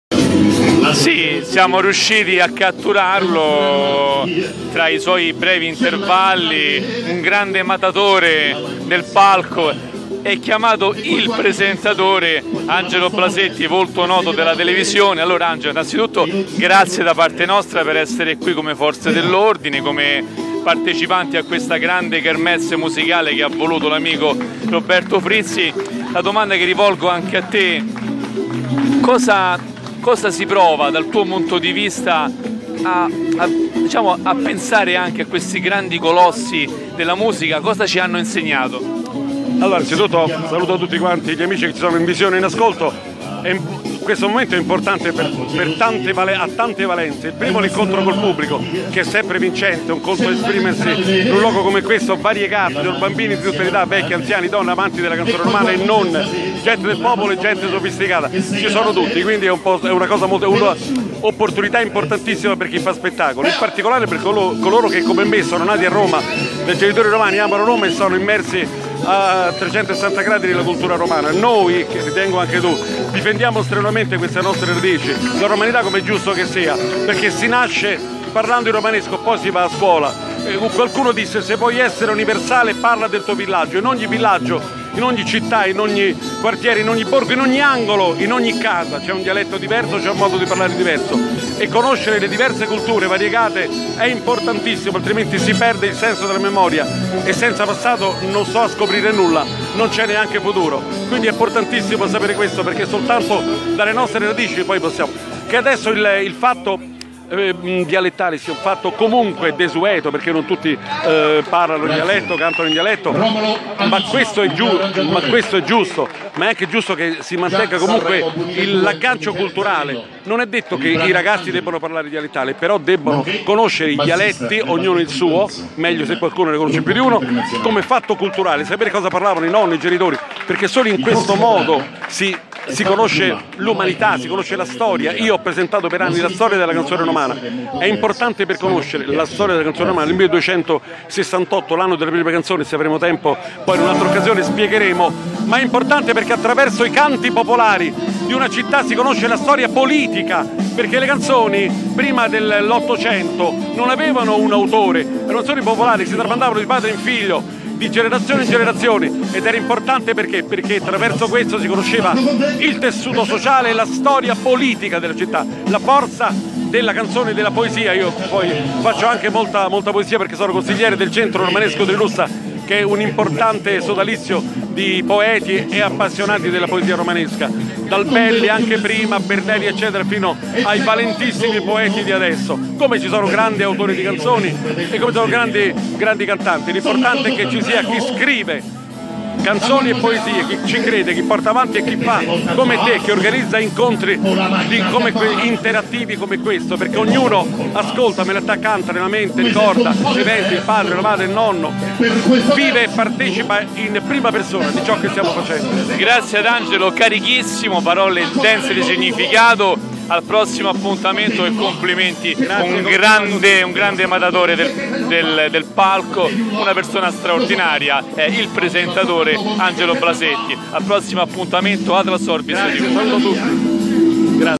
Diretta Atlasorbis, Canta Roma 2018, Galà della Musica e Poesia Romana, con Angelo Blasetti
Diretta Atlasorbis, Canta Roma 2018, Galà della Musica e della Poesia Romana, con Angelo Blasetti

Straordinaria esibizione di grandi artisti che interpretano la canzone romana quello di oggi, a partire dalle ore 15.30, all‘Oasi Park, a via Tarquinio Collatino 56 (zona Cinecittà – Don Bosco), il CANTA ROMA II^ edizione, evento musicale in omaggio al ricordo di Gabriella Ferri, Lando Fiorini, Claudio Villa, Alvaro Amici e Franco Califano

Ideatore e conduttore dell’evento il celebre Roberto Frizzi, speaker di Elleradio (88,100 FM) e attivista in volontariato presso l’Associazione ARGOS Forze di POLIZIA, mattatore sul palco insieme all’attore Angelo Blasetti e Gianluca Guerrisi

Con il patrocinio della CONSAP – Confederazione Sindacale Autonoma di Polizia  – Segreteria Provinciale di Roma, TV Partner Retesole canale 87 DGT e Radio Partner Elleradio 88, 100 FM, giornali partner Atlasorbis e Oasi Park News

Roma, Oasi Park, sabato 12 Maggio 2018

Video di Samuel Gallone
Intervista di Gianluca Guerrisi